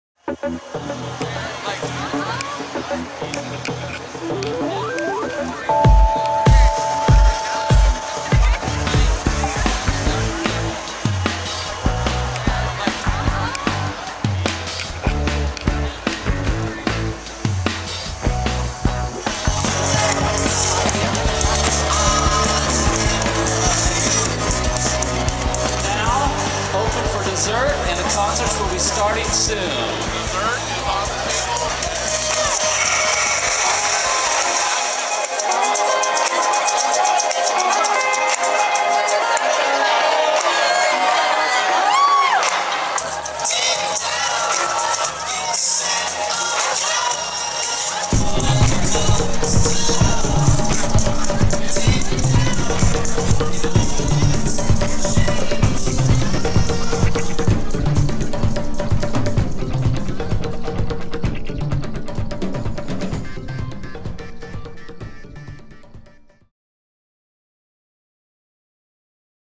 SURPRISE! It Flies! Event Reveal - Flying Balloon Wall by TLC Creative
Special event Reveal sequence: TLC created this reveal at a special event at LA Live - as this seemingly colorful decor suddenly flies away revealing a walkway to the next area of the event. Contact TLC Creative for more information - visit TLCisCreative.com